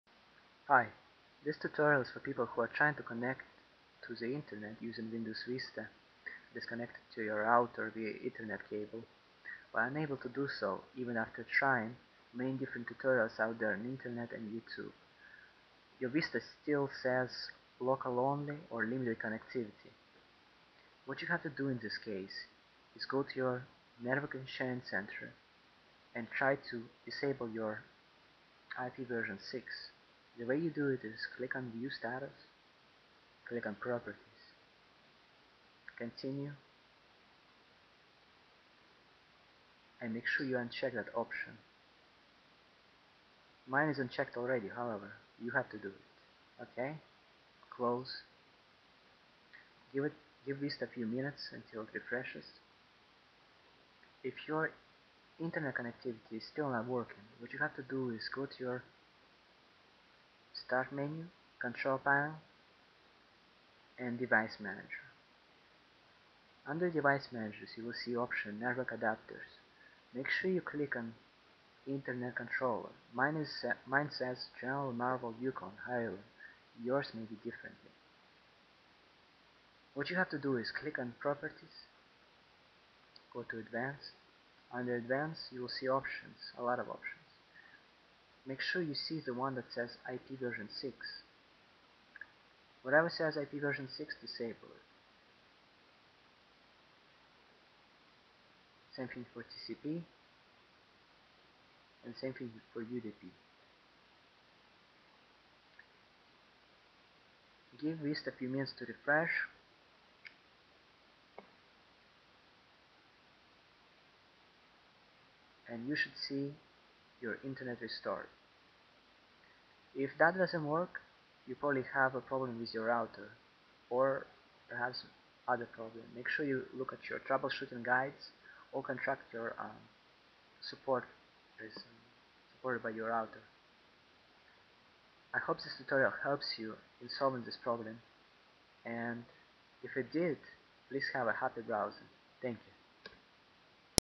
How to Fix "Local Only" or "Limited Connectivity" in Vista
*My first video, so take it easy on me @_@*

REASON:
This tutorial is for the people who ALREADY tried other tutorials to connect to the internet but still have your vista showing local only or limited connectivity, even thought your other windows xp based computers connect without a problem to your router.

This solution solved my problem, so I thought I'd share it.

WHAT THIS TUTORIAL DOES:
step by step instructions to disable IPv6 and so that only IPv4 will function, allowing Vista to connect to older or routers such as NETGEAR

MAKE SURE YOU REMEMBER ALL PREVIOUS  SETTINGS BEFORE USING THIS METHOD!!!

WAS THIS TUTORIAL USEFUL?
If you REALLY, REALLY liked this tutorial and want give appreciation to poor college student , I ACCEPT !!! *_*